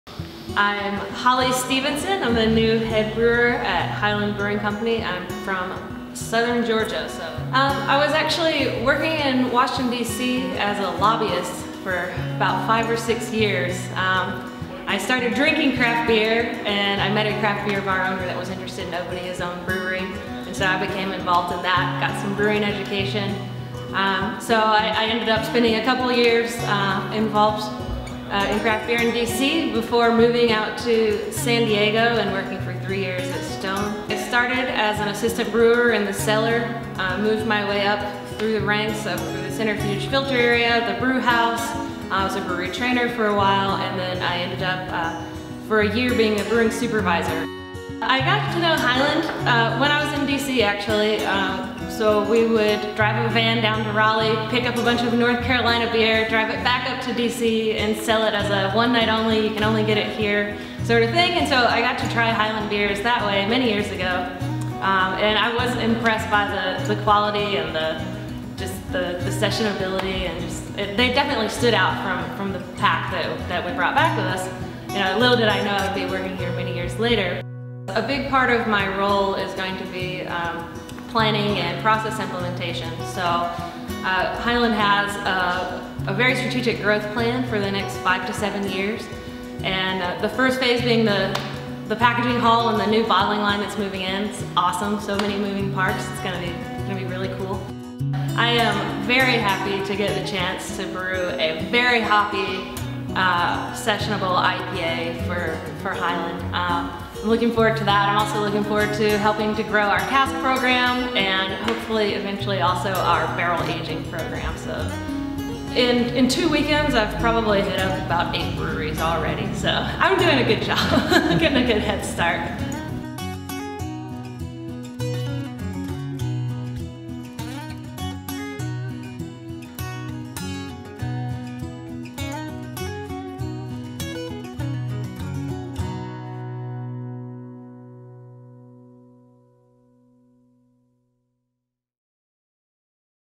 Meet Hollie Stephenson, Highland Brewing's new head brewer
This week, the Asheville Ale Trail invites you to our very first event - a Scouting Party! Scouting Parties are new feature at the Asheville Ale Trail, and are designed to give you a chance to learn more about our local breweries and meet some of the people behind the scenes (tanks?) that work hard to bring you some of the best craft beer in the world.

Every month, we'll have a new Scouting Party for you to check out and enjoy.

This week, we invite you to come meet Hollie Stephenson, the new head brewer at Highland Brewing Co., and learn more about what's happening with Highland's major new expansion.

The Scouting Party starts at 5:30, and Hollie will begin a brief talk at 6:00.  Afterwards, hang out and mingle with Highland Staff, who will be on hand to talk about the Brewery, new beers, and what to expect for the next 20 years!

Cheers!